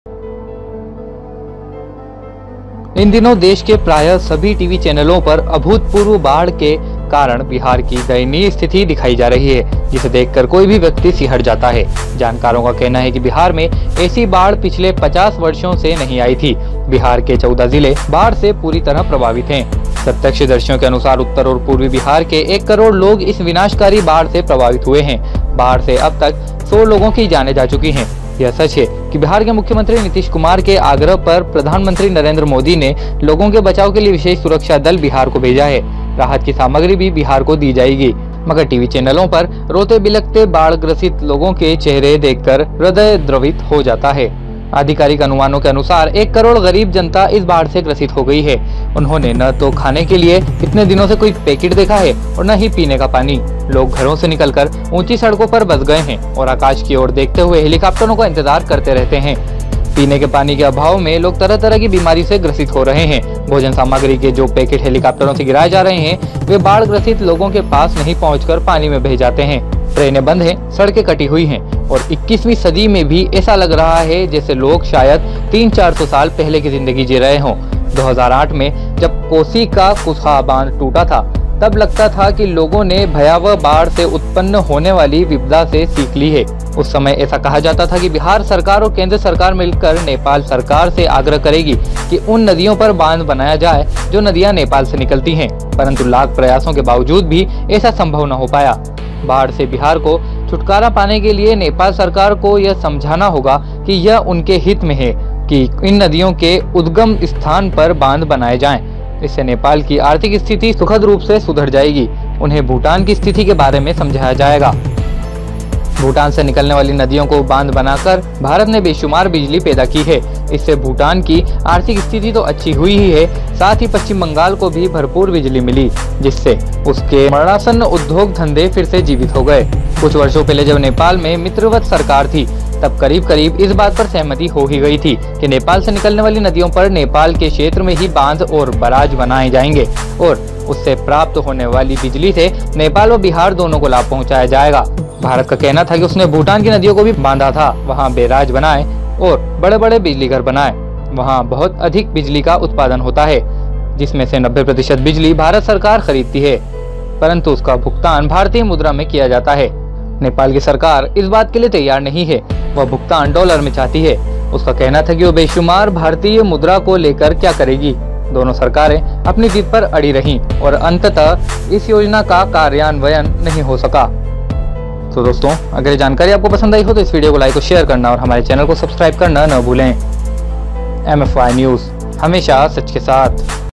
भारत की एक शर्त जो नहीं बनने देती नेपाल में बांध और डूब जाता है बिहार
इन दिनों देश के प्राय: सभी टीवी चैनलों पर अभूतपूर्व बाढ़ के कारण बिहार की दयनीय स्थिति दिखाई जा रही है, जिसे देखकर कोई भी व्यक्ति सिहर जाता है। जानकारों का कहना है कि बिहार में ऐसी बाढ़ पिछले पचास वर्षों में नहीं आई थी। बिहार के 14 जिले खासकर, अरहरिया, सुपौल, किशनजंग, कटिहार, पूर्णिया, पूर्वी तथा पश्चिमी चंपारण, दरभंगा और सीतामढ़ी जिले बाढ़ से बुरी तरह प्रभावित हैं। प्रत्यक्षदर्शियों के अनुसार उत्तर और पूर्वी बिहार के एक करोड़ लोग इस विनाशकारी बाढ़ से प्रभावित हैं। बाढ़ से अब तक 100 लोगों की जान जा चुकी है